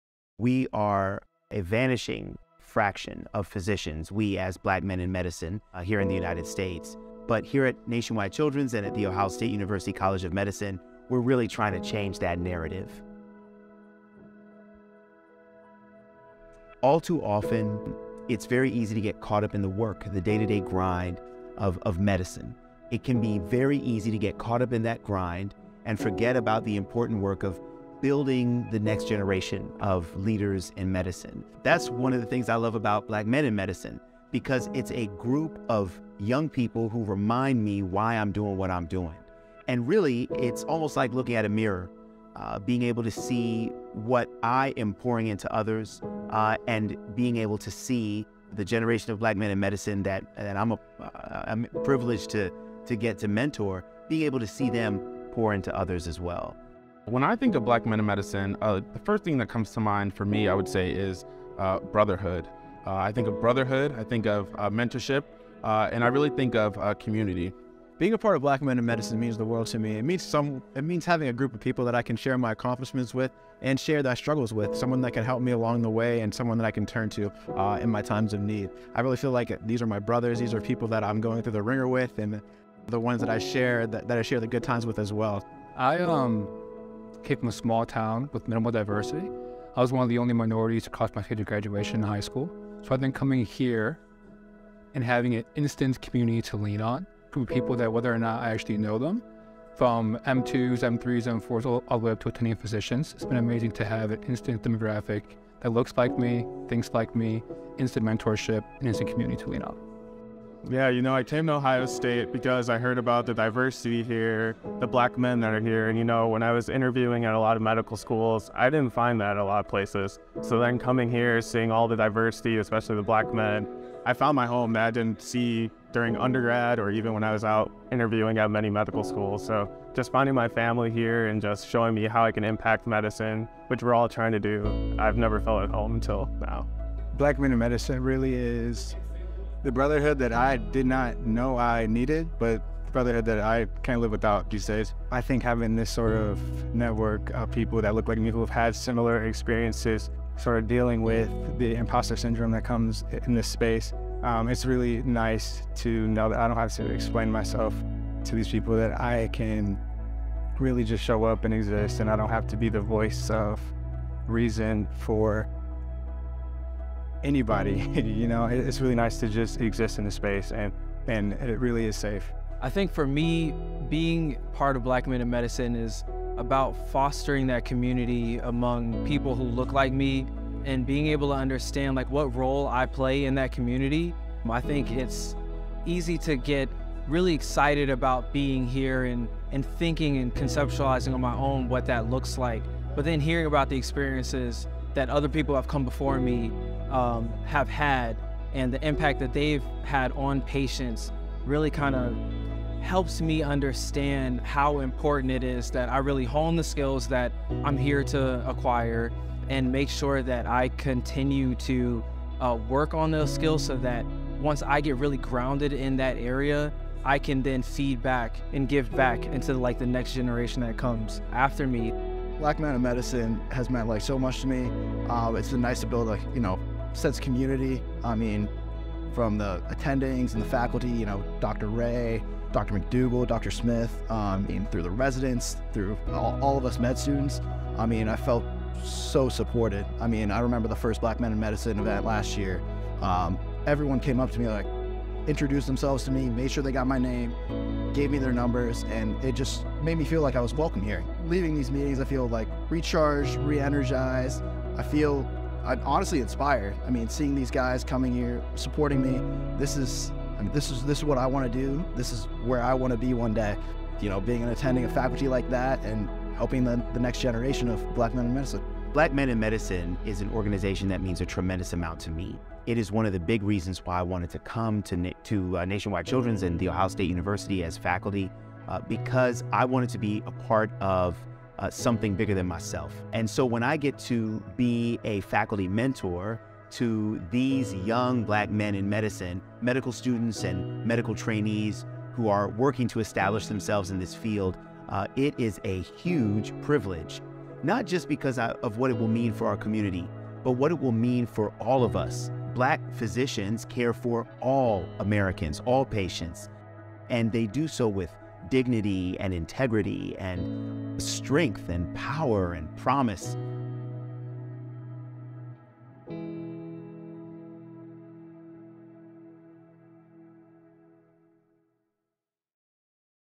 Black Men in Medicine encourages young black men to enter the field | Ohio State College of Medicine
The mission of Black Men in Medicine at The Ohio State University College of Medicine is to demonstrate the excellence put forth by black male physicians and rising students in the medical field as a means to increase the recruitment of black males in medicine.

They also aim to provide a secure platform for minority males to openly discuss the difficult topics faced as minorities in medicine.